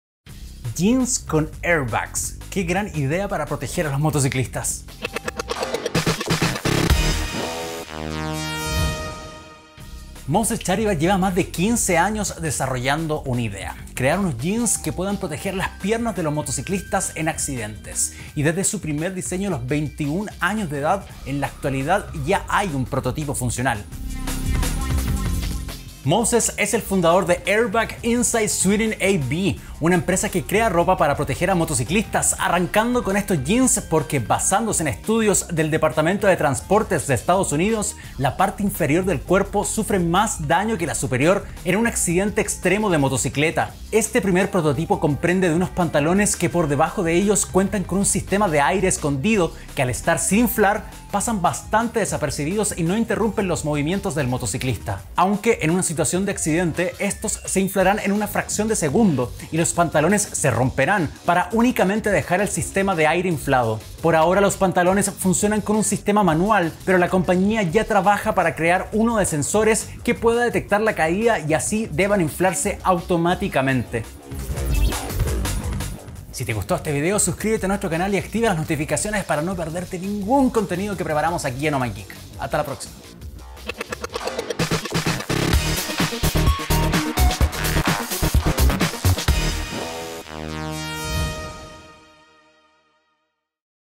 Prototipo de Jeans con Airbags buscan cuidar a motociclistas en accidentes
Moses Shahrivar, con su empresa Airbag Inside Sweden AB, quiere proteger a los motociclistas de accidentes. Y por años ha estado trabajando en unos jeans que incorporan un sistema de airbags que se inflan en una fracción de segundo. Actualmente ya cuenta con un primer prototipo funcional.